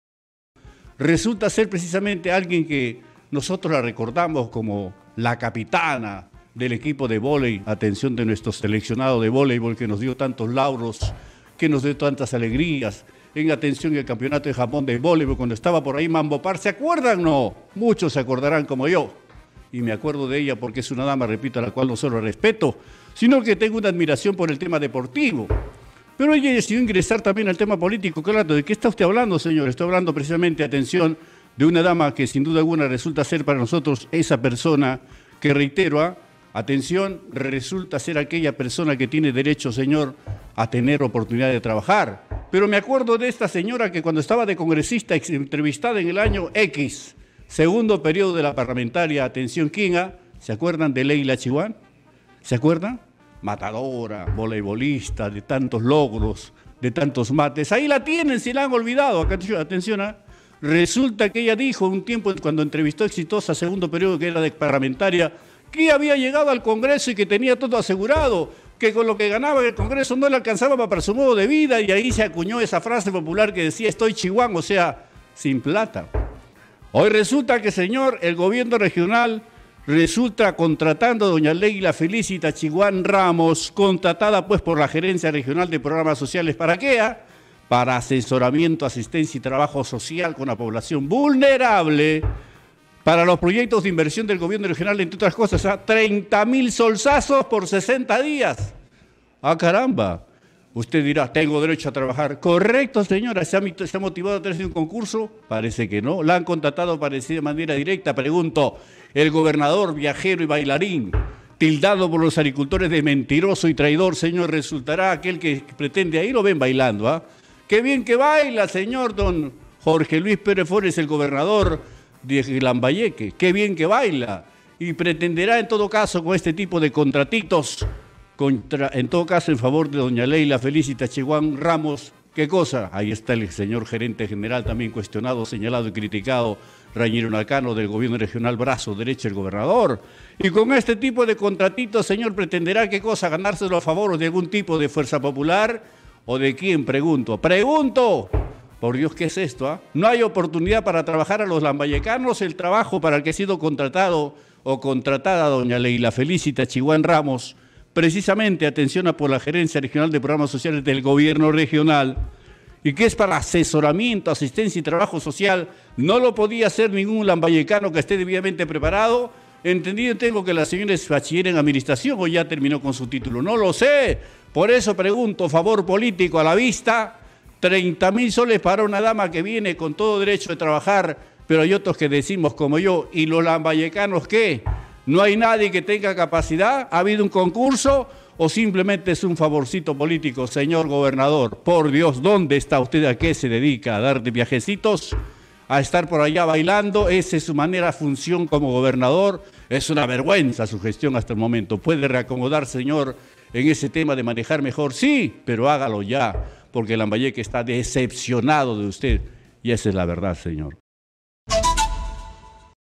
🔴🔵Leyla Chihuán: Gore paga S/30 mil a excongresista para trabajar en programa social
El Gobierno Regional de Lambayeque contrató a la exparlamentaria Leyla Chihuán con un pago de S/30 mil por 60 días para asesorarlos en un programa social dirigido a personas vulnerables.

Exitosa Perú
"Exitosa: La voz que integra al Perú"

Corporación Universal